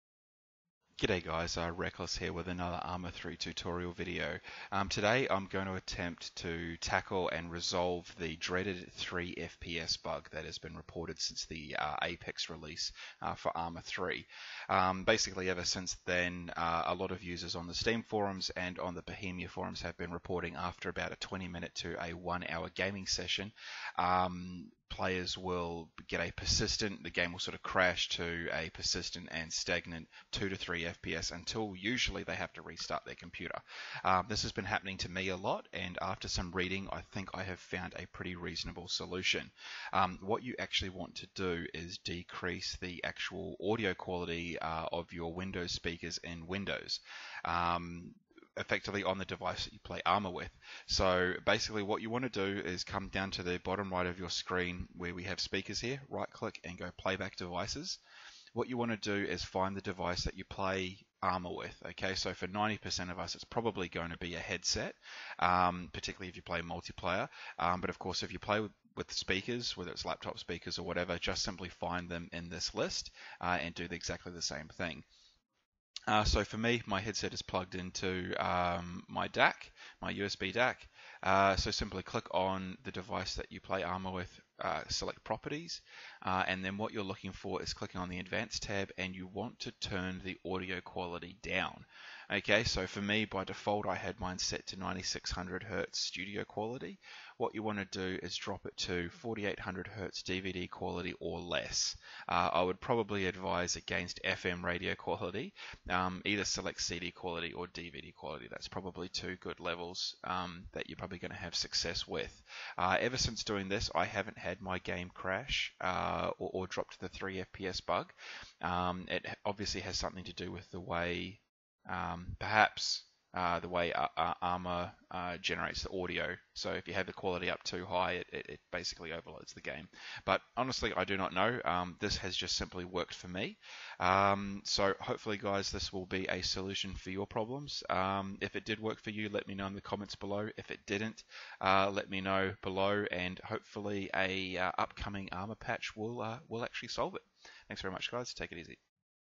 ArmA 3 - How To Fix The 3FPS Bug, How To Fix Game Crashes
A Simple guide on how to decrease the chances of your ArmA 3 from crashing or dropping to the dreaded 3fps bug since the release of ArmA 3 Apex.